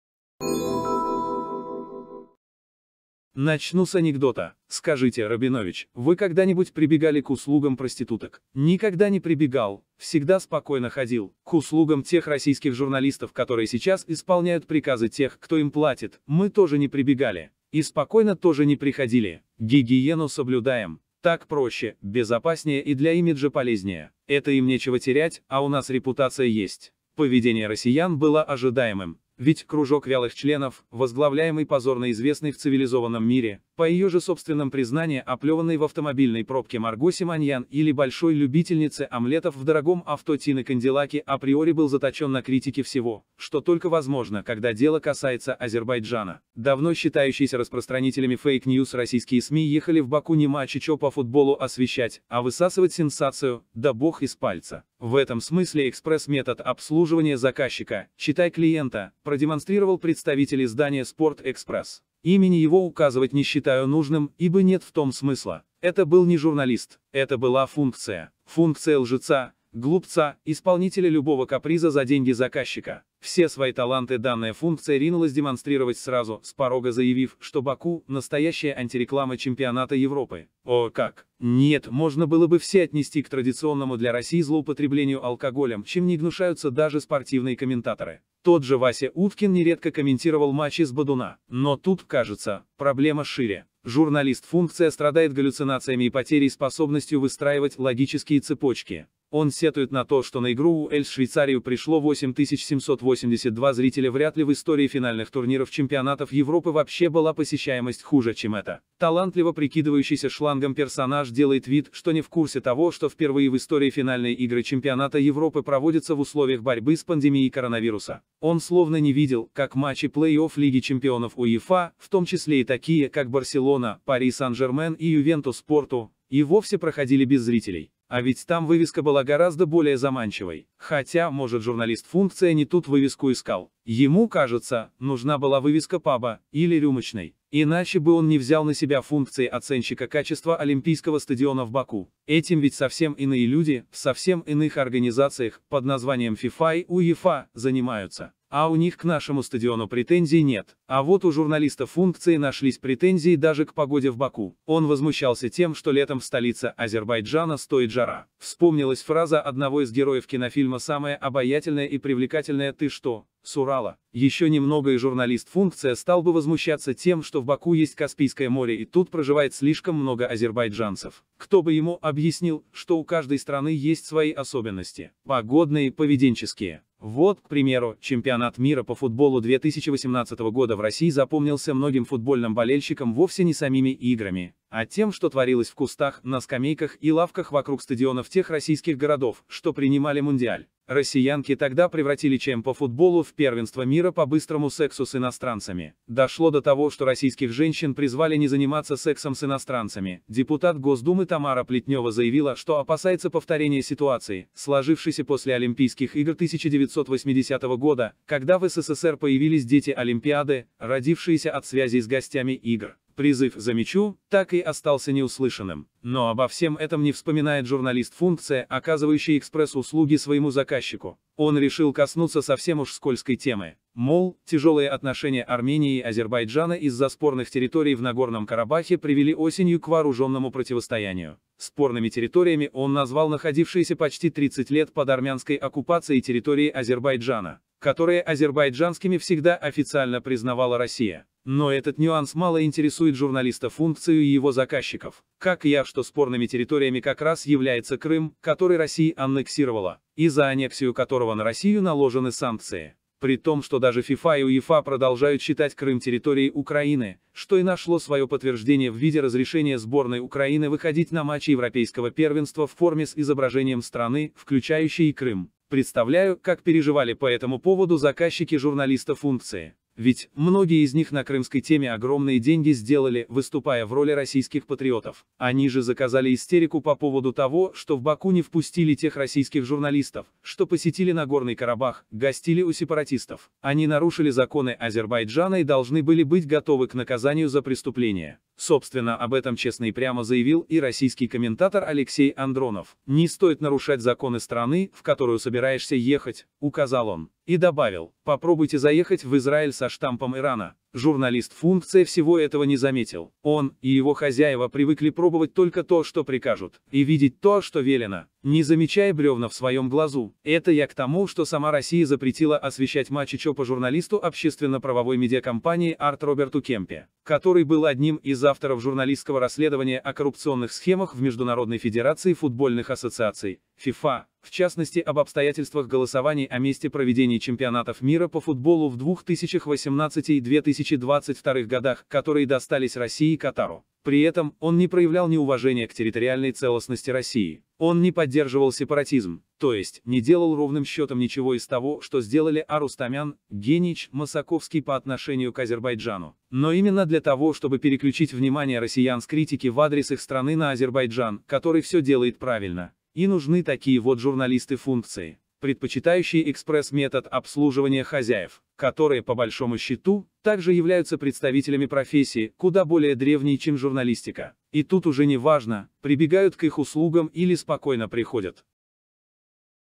Экспресс глупость от российского "журналиста" кто и как удовлетворяет Марго и Тину
14 июнь. 2021 г. Новость дня. Азербайджана и Армении. КАРАБАХ